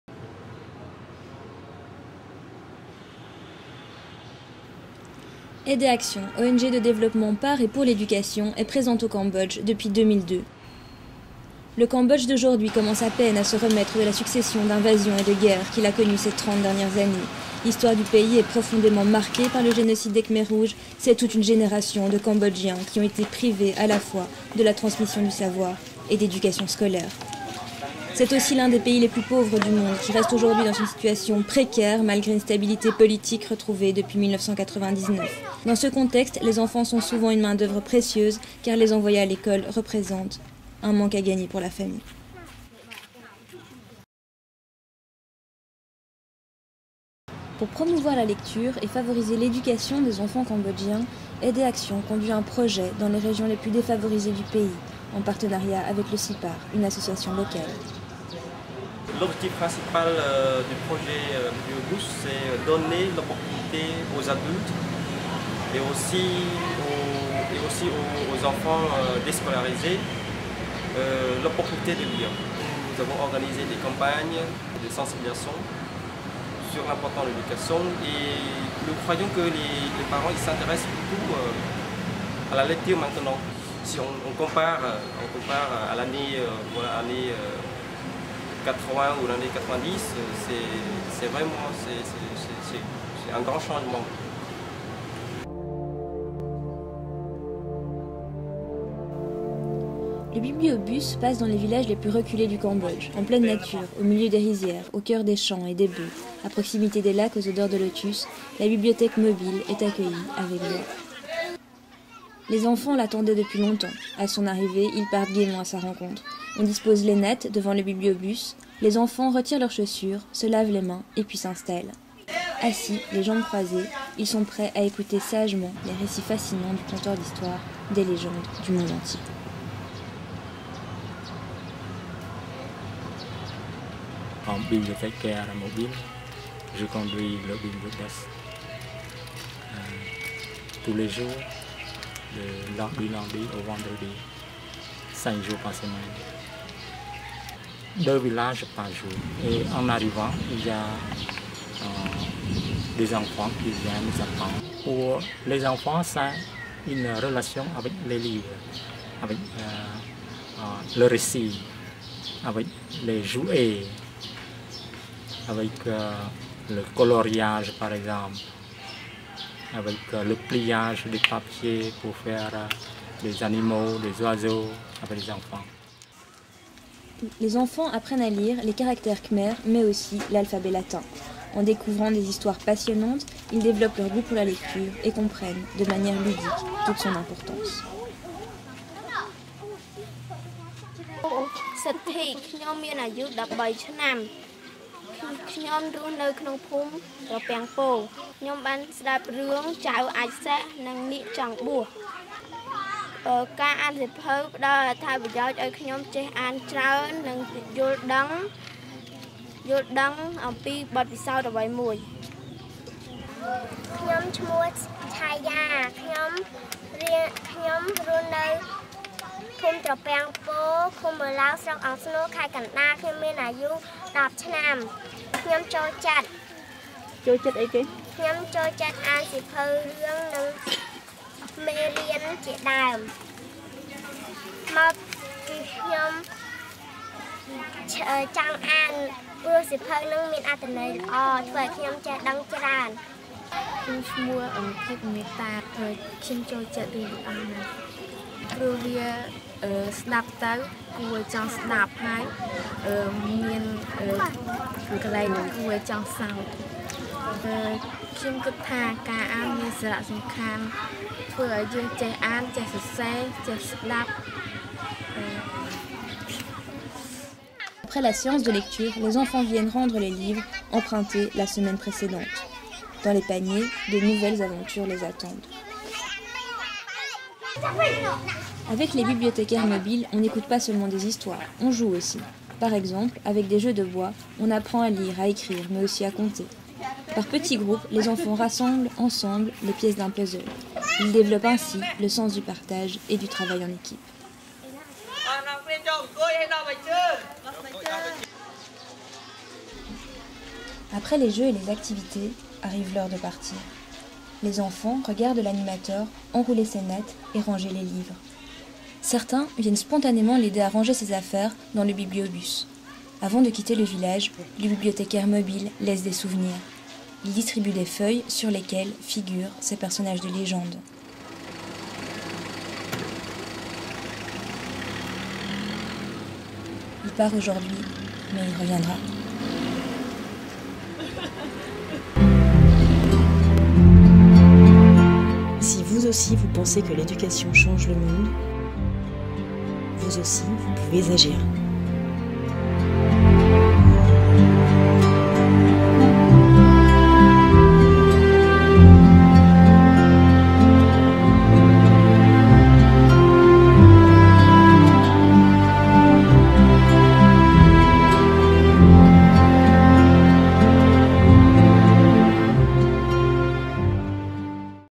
A la rencontre des conteurs de légendes du Cambodge
Aide et Action, présente au Cambodge depuis 2002, contribue à améliorer l'éducation dans un pays dévasté par une succession d'invasions et de guerres et qui, malgré une stabilité retrouvée depuis 1999, reste dans une situation de précarité. Les bibliothécaires mobiles sillonnent les villages du pays pour promouvoir la lecture dans les régions les plus défavorisées.